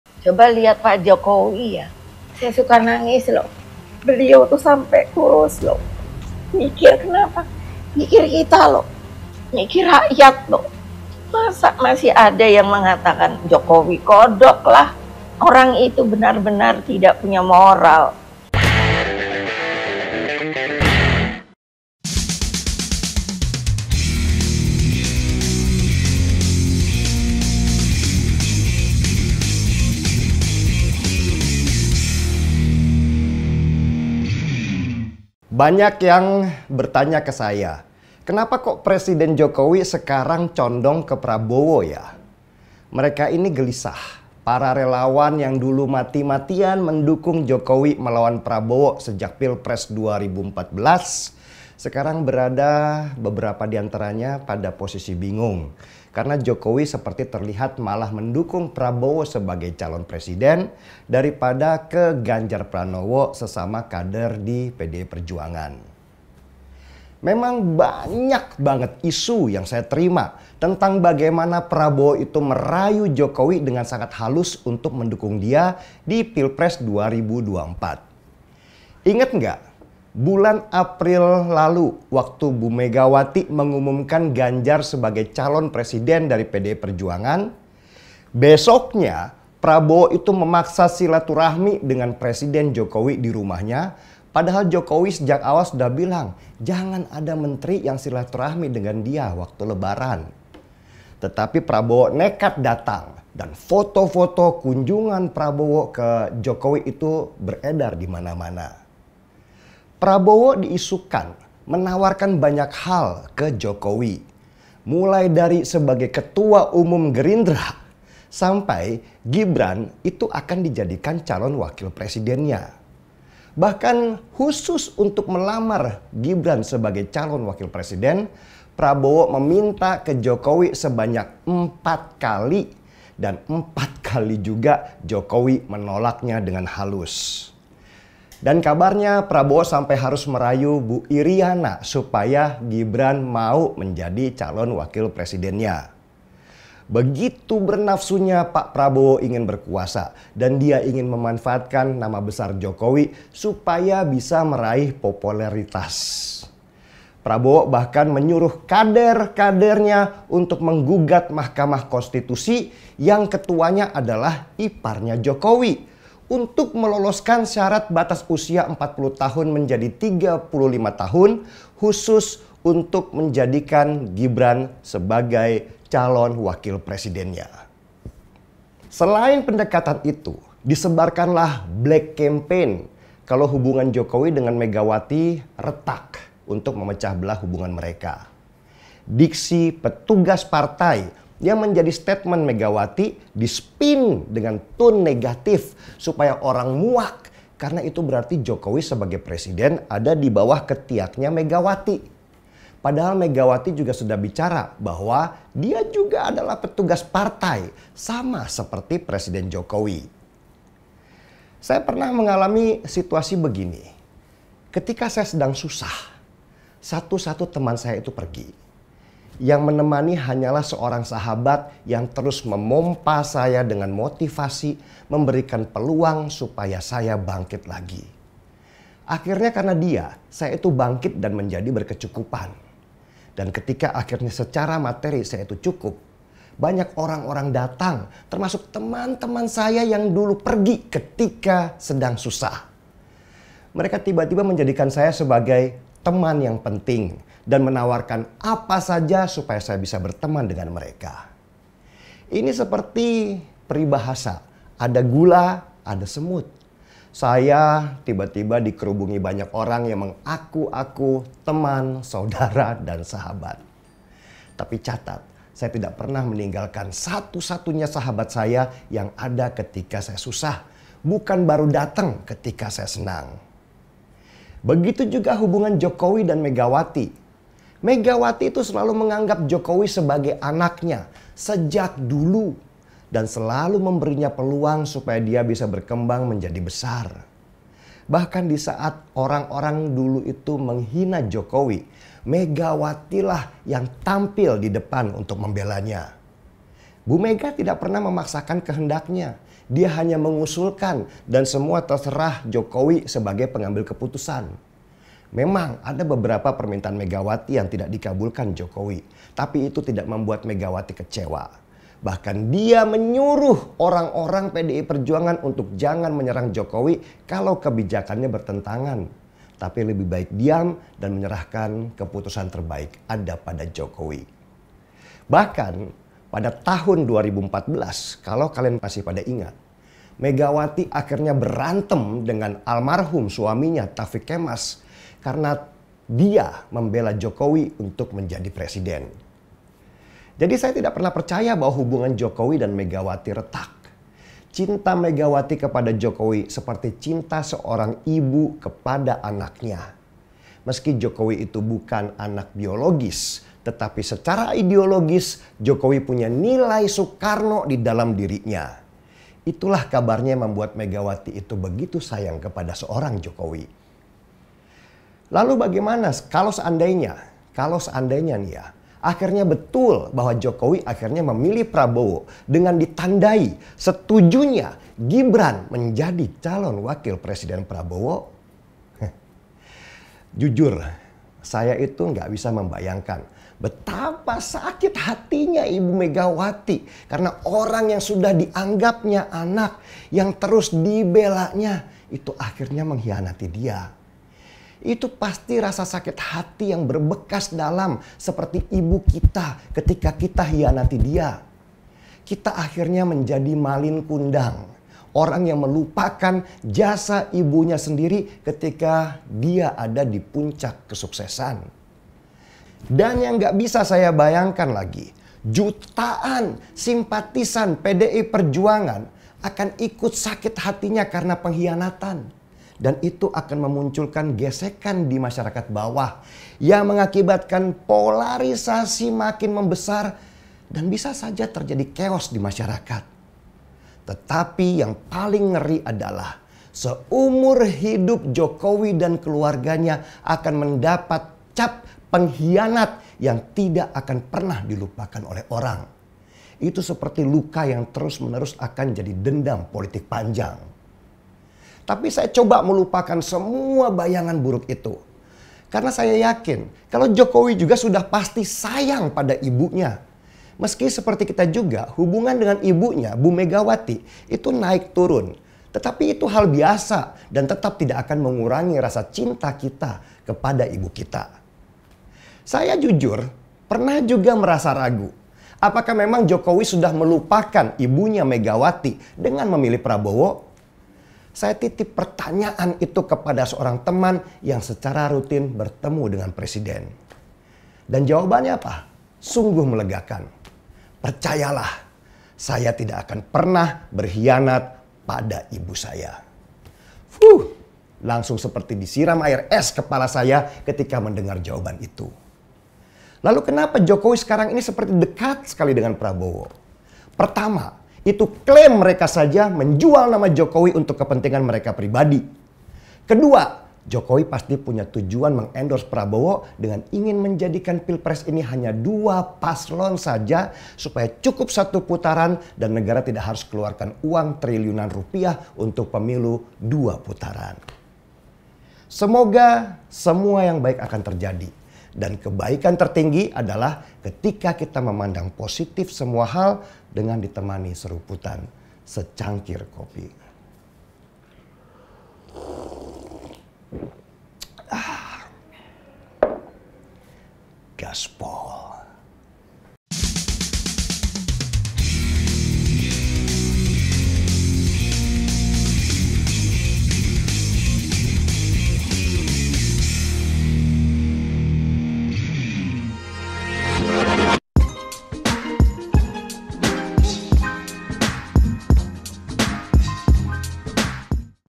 Denny Siregar: JOKOWI & KELUARGANYA BUKAN PENGHIANAT ‼️ (Gaspol #227)
Jokowi tak akan menghianati Megawati !!